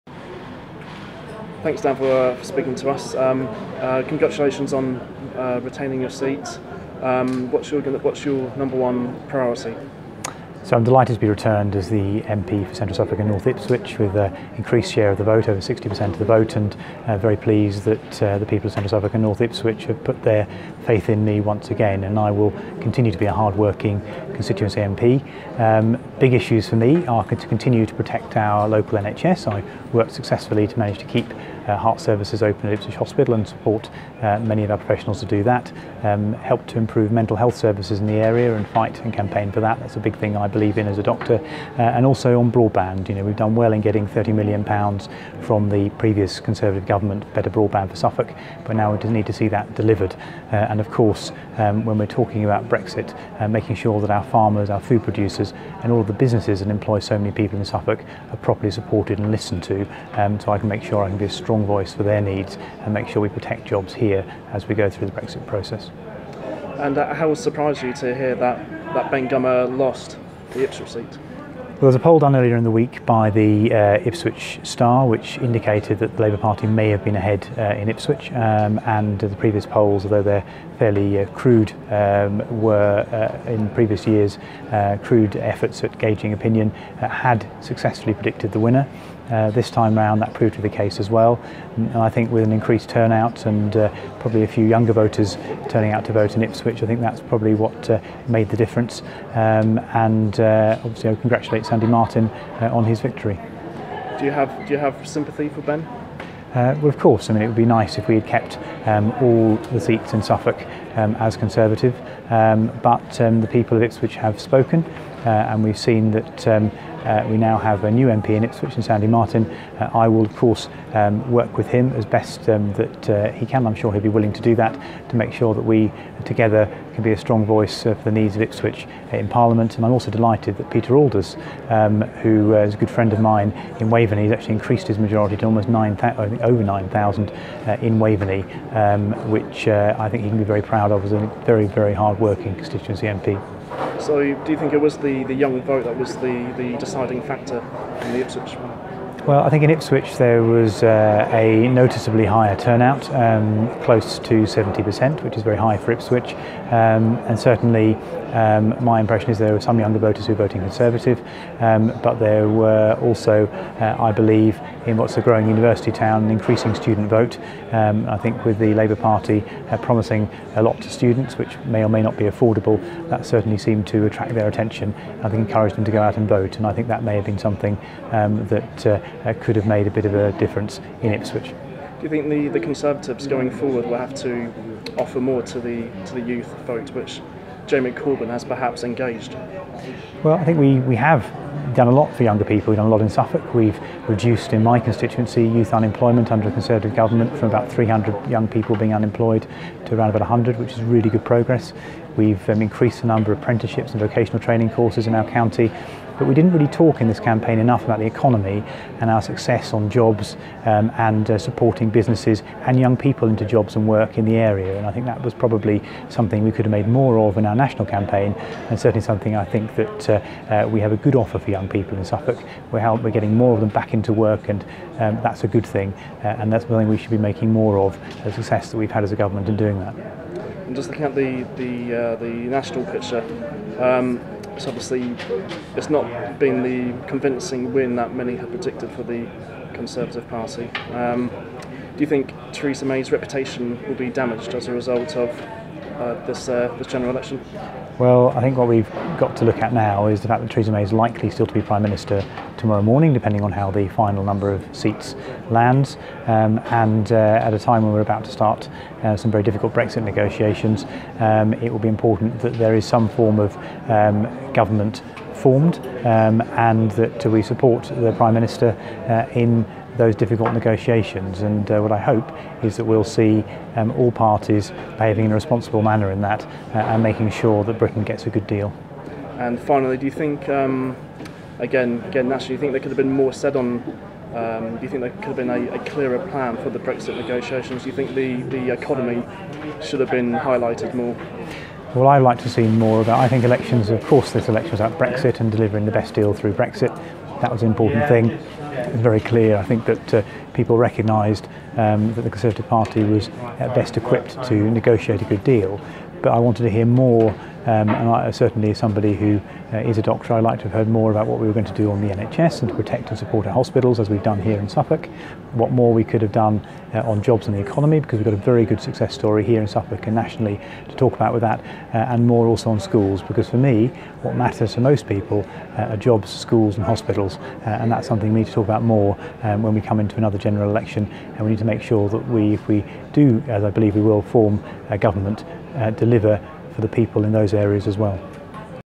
Dr Dan Poulter interview after General Election 2017 win
Conservative Dr Dan Poulter speaks after holding his Central Suffolk and North Ipswich seat